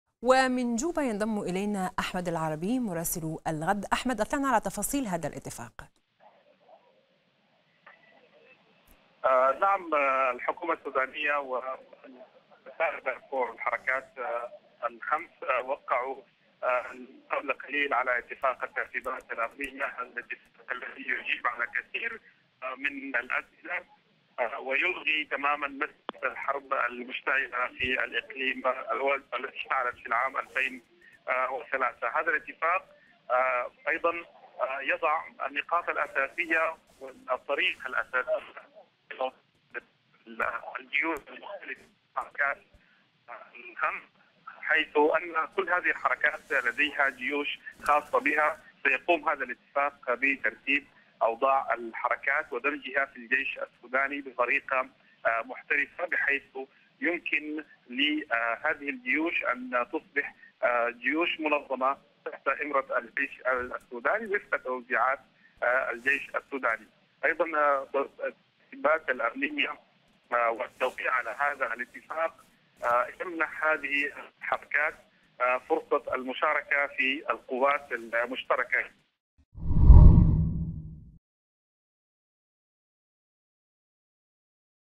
توقيع الاتفاق الأمني بين الخرطوم ومسار دارفور.. إليك التفاصيل
أفاد مراسل الغـد بأن الحكومة السودانية والجبهة الثورية مسار دارفور وقعتا اتفاق الترتيبات الامنية.
وينص الاتفاق على دمج قوات الحركات المسلحة بمسار دارفور في الجيش السوداني في غضون تسعة وثلاثين شهرا من توقيع الاتفاق النهائي. ويعتبر اتفاق الترتيبات الأمنية  آخر ملفات التفاوض ضمن ثمانية تم التوقيع على سبعة منها بالأمس، وذلك قبيل التوقيع الشامل بالاحرف الاولى على الاتفاق النهائي للسلام بين الحكومة السودانية والجبهة الثورية الاثنين المقبل.
المزيد من التفاصيل يتحدث مراسل الغد من جوبا احمد العربي مع الإعلامية منى بلهيم عبر النشرة الإخبارية على شاشة قناة الغد.